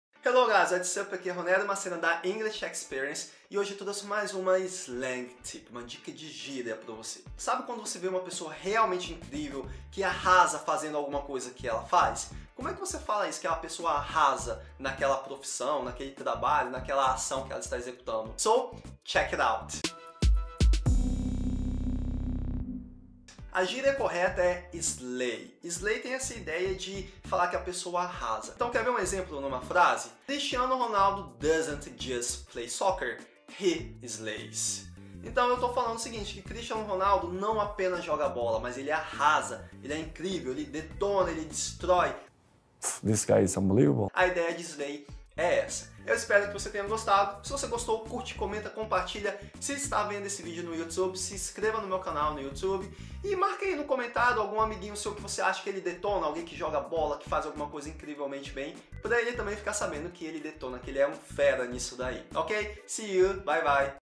Slay - Se alguém falar isso para você fique MEGA FELIZ!
Essa gíria Slay significa "incrível" mas varia conforme o contexto. Assista ao vídeo para entender.